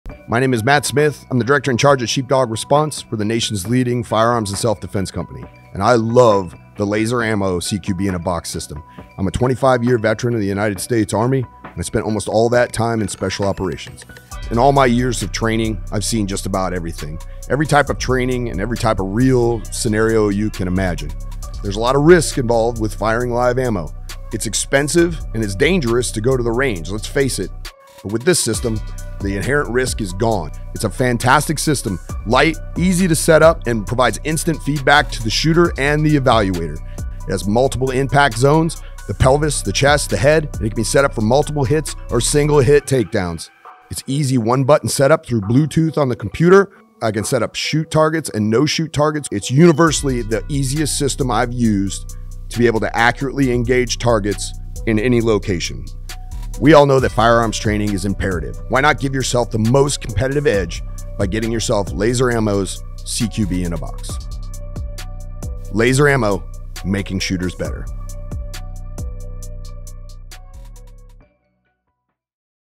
Laser Ammo's CQB in a Box by Matt Smith of Sheepdog Response
Matt Smith shares his review and demo of the CQB in a Box, the importance of dry fire training and close quarters training.

TRANSCRIPT:

My  name  is  Matt  Smith.  I'm  the  director  in  charge  of  Sheepdog  Response  for  the  nation's  leading  firearms  and  self-defense  company.  And  I  love  the  Laser  Ammo  CQB  in  a Box  system.  I'm  a  25 -year  veteran  of  the  United  States  Army  and  I  spent  almost  all  that  time  in  special  operations.

In  all  of my  years  of  training,  I've  seen  just  about  everything.  Every  type  of  training  and  every  type  of  real  scenario  you  can  imagine.  There's  a  lot  of  risk  involved  with  firing  live  ammo.  It's  expensive  and  it's  dangerous  to  go  to  the  range. Let's  face  it,  with  this  system,  the  inherent  risk  is  gone.

It's  a  fantastic  system,  light,  easy  to  set  up  and  provides  instant  feedback  to  the  shooter  and  the  evaluator.  It  has  multiple  impact  zones: the  pelvis,  the  chest and  the  head,  and  it  can  be  set  up  for  multiple  hits  or  single  hit  takedowns.  It has  easy  one  button  set  up  through  Bluetooth  on  the  computer.  I  can  set  up  shoot  targets  and  no-shoot  targets.

It's  universally  the  easiest  system  I've  used,  to  be  able  to  accurately  engage  targets  in  any  location.  We  all  know  that  firearms  training  is  imperative.  Why  not  give  yourself  the  most  competitive  edge  by  getting  yourself  Laser  AMMO's  CQB  in  a  Box.

Laser  AMMO, Making Shooters Better.